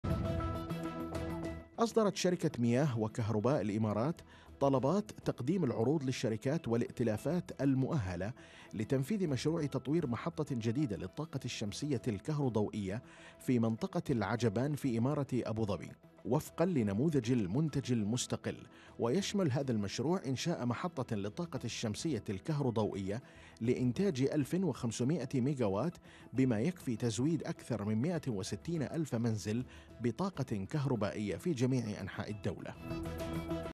"مياه وكهرباء الإمارات" تصدر طلبات لمشروع تطوير محطة العجبان للطاقة الشمسية
تقرير : ياسر عبد الله